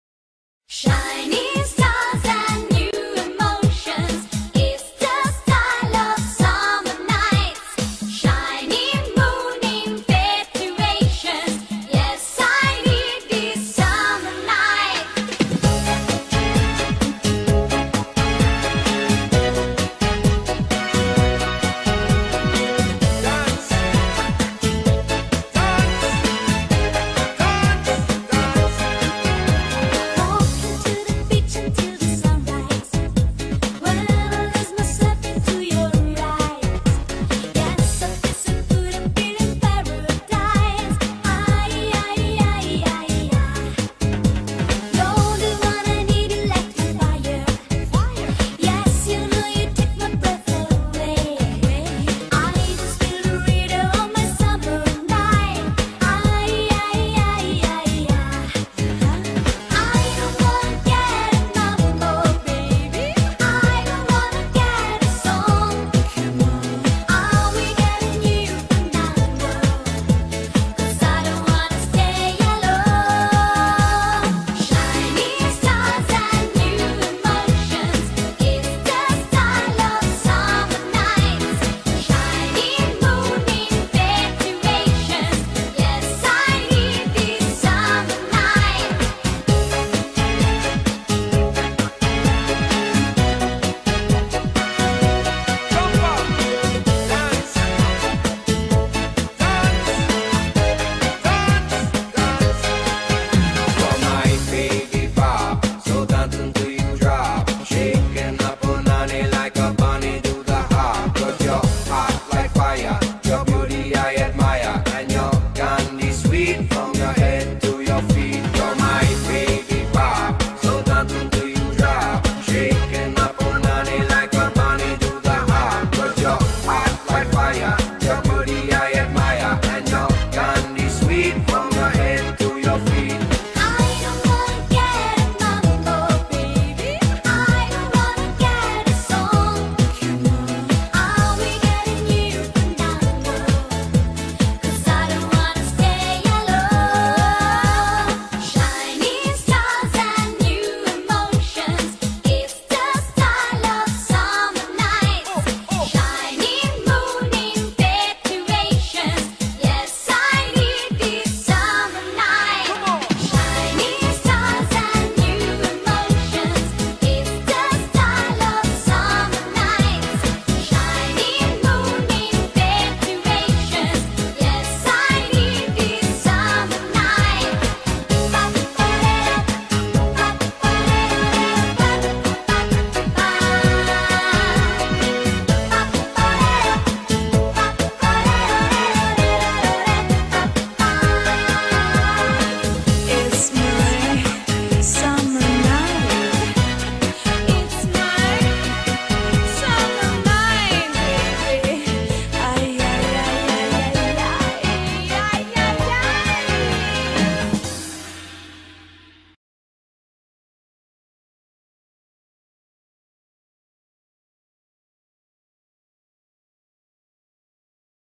Cha Cha Cha 2021 - Line Dance (Demo by Linda's Dance Kirana)
Count : 32
Wall : 4

Choreographed by Hantos Djay
Dance by Linda's Dance Kirana